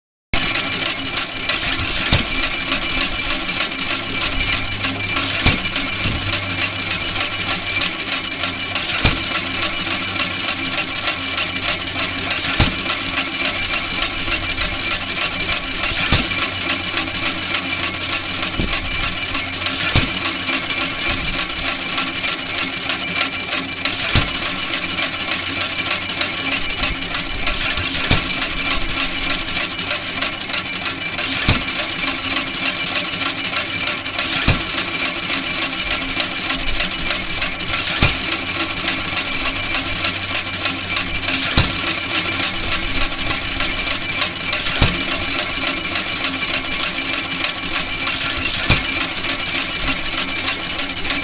Economy Stationary Engine
A 1917 Economy hit and miss stationary gas engine.